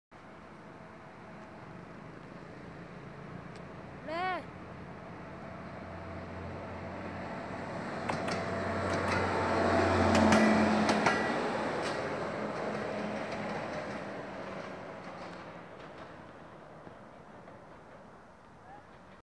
Μανούβρες 1
Η adtrαnz,επικεφαλής της τοπικής αμαξοστοιχίας απο Βόλο,εκτελεί μανούβρες,για να μπει στην άλλη πλευρά της αμαξοστοιχίας.